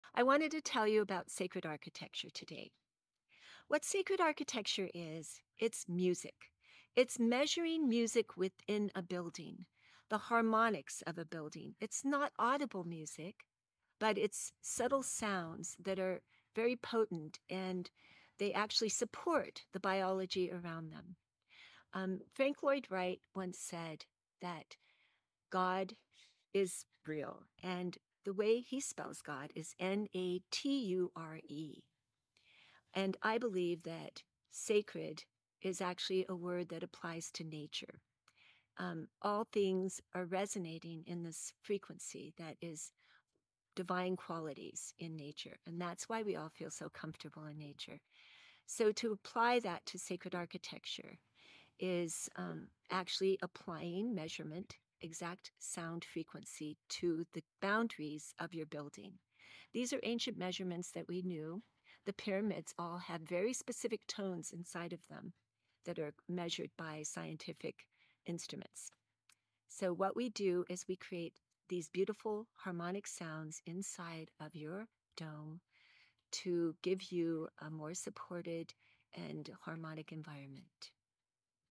Harmonics and Importance of Dome Architecture
Discover the unique harmonics and environmental benefits of dome architecture, designed to enhance both structural integrity and natural living. Learn why domes are more than just shelters—they’re the future of sustainable design. Ready to experience it firsthand? Call Pacific Domes today to explore our innovative dome solutions for your next project!